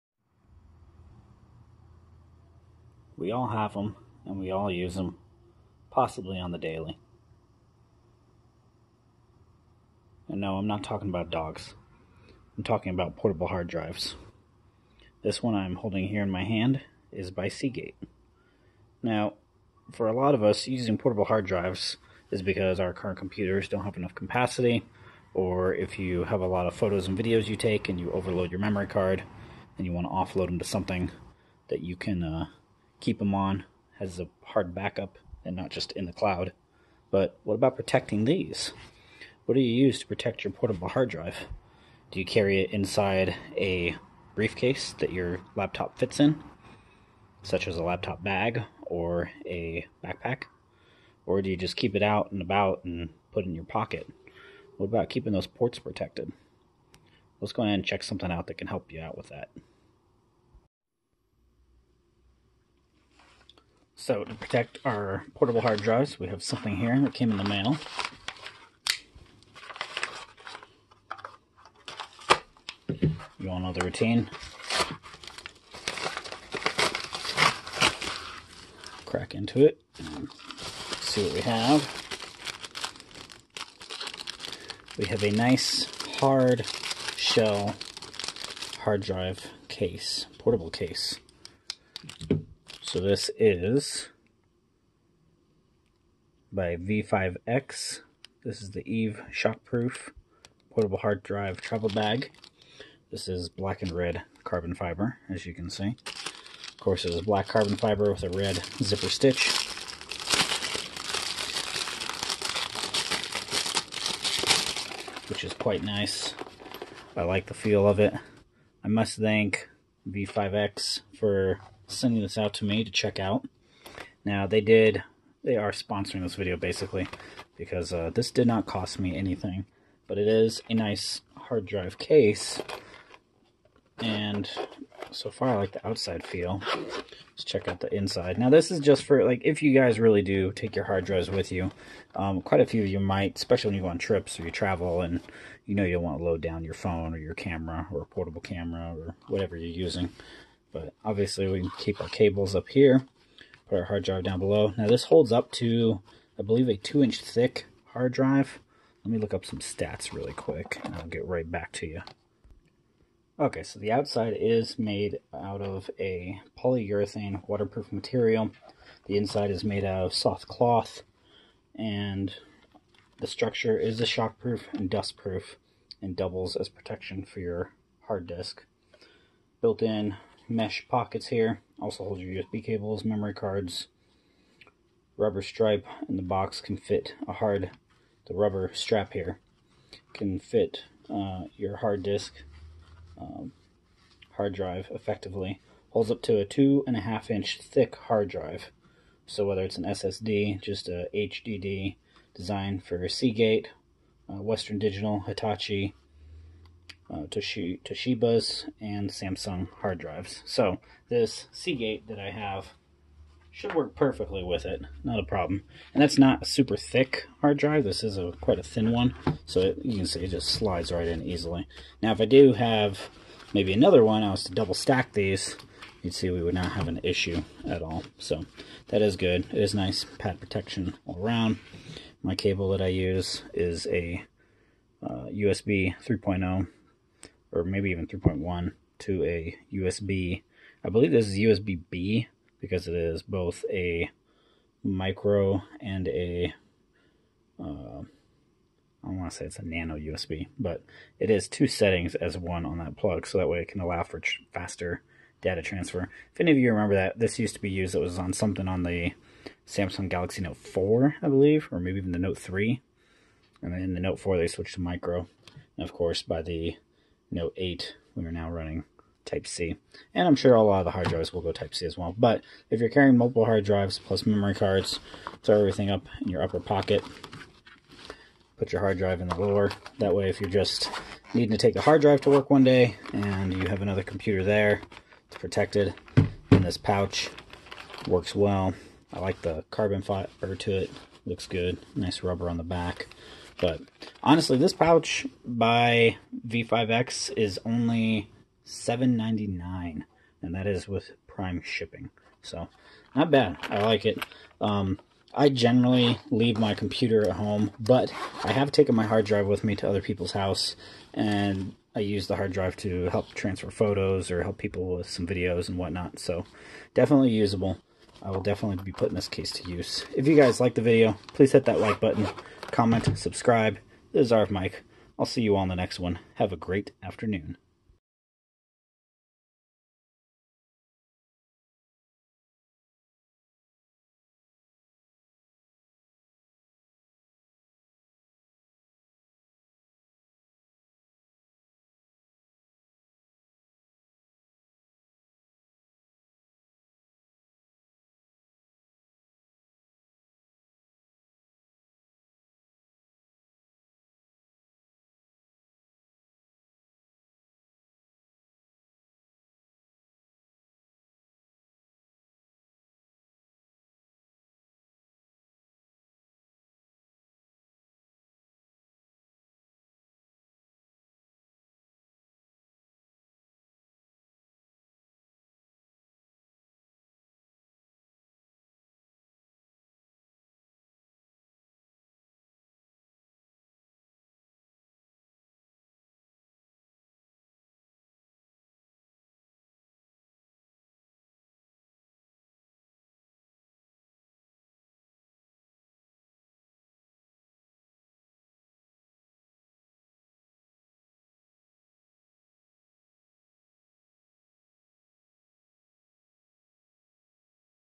Carbon fiber portable hard drive Case
If you need a hard drive case that won't brake the bank...
check this one out..